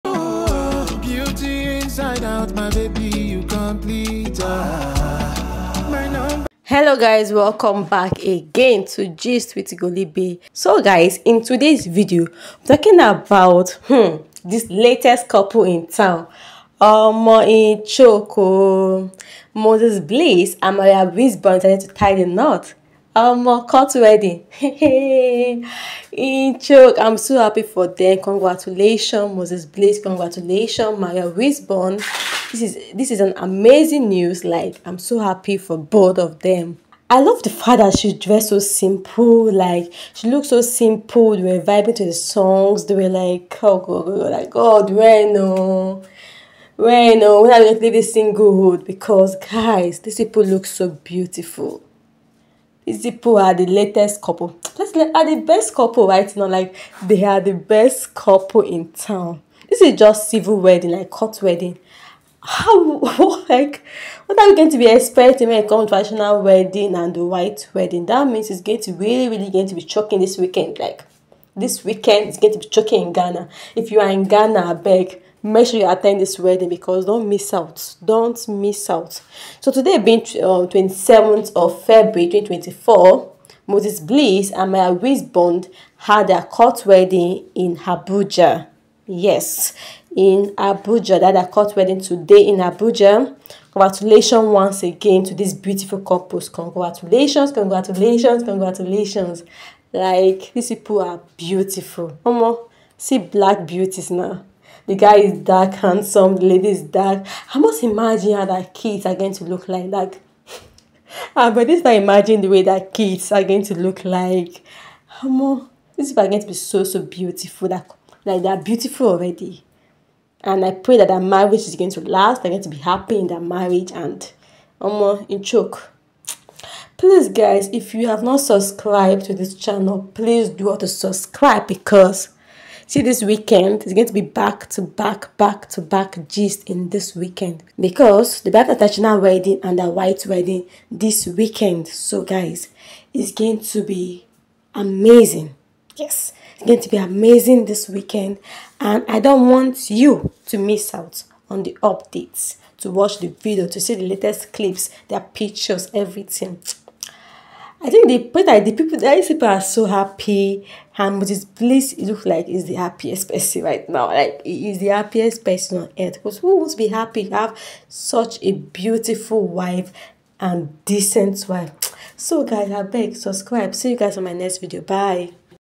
Moses Bliss and Wife Marie Civil Marriage (Full Video) Exchange of Vows
Hello everyone in today's videos we will be talking about Moses Bliss and Wife Marie Wiseborn Civil Marriage (Full Video) Exchange of Vows